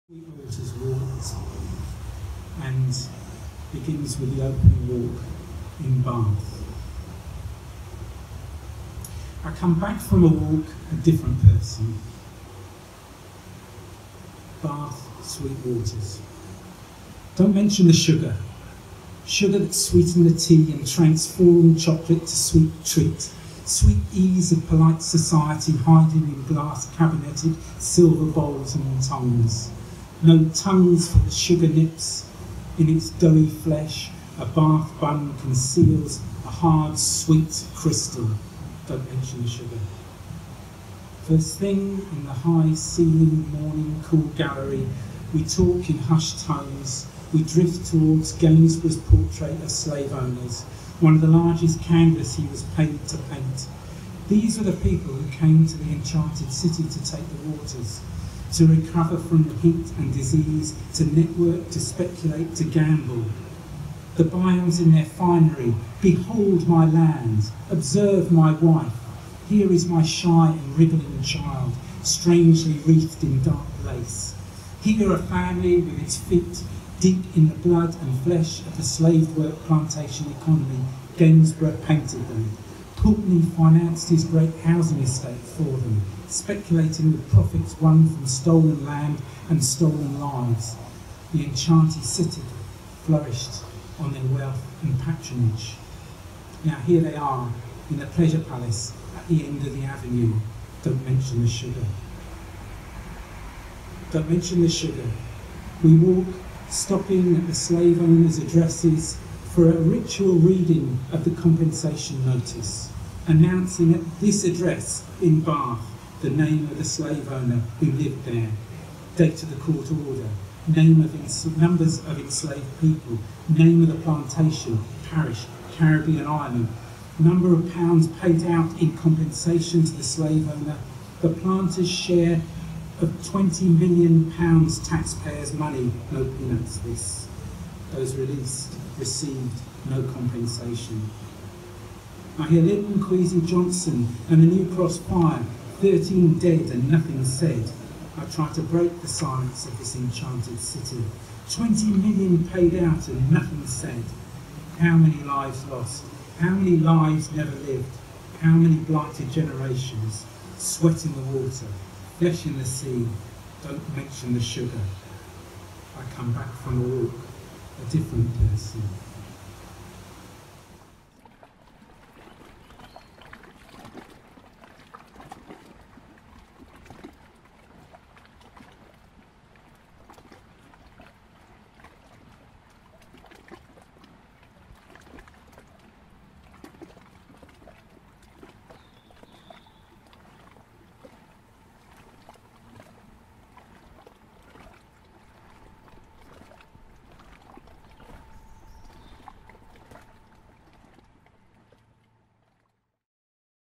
Dont Mention the Sugar
Performance prose piece recorded live at Sweet Waters:Soundings
Richard White at Saltford Brass Mill